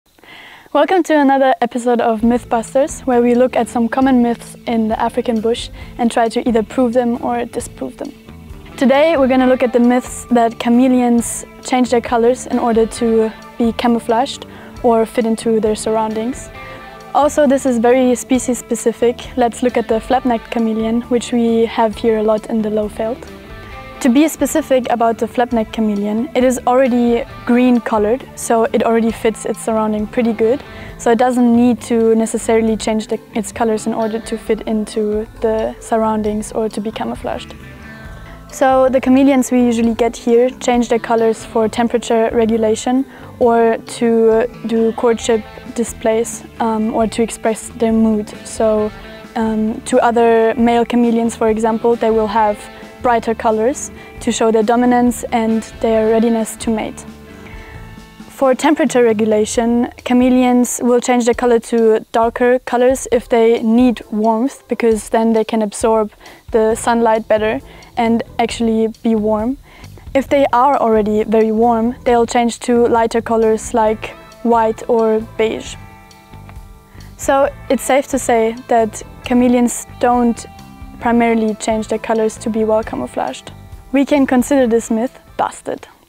Do chameleons change colour to match their surroundings? l Mythbusters - Ep 26 | EcoTraining
We're featuring the 26th episode of MythBusters. In this series, we examine common beliefs about African animals that may need revision.

Penelope Gussman, an EcoTraining student, examines the belief that some people hold that chameleons change their colours to blend into their surroundings. Let’s take a closer look to determine if this is true or if it is a myth that gets busted!

Have you heard any myths you'd like us to debunk in our series?

Follow the EcoTraining journey on social media @EcoTraining.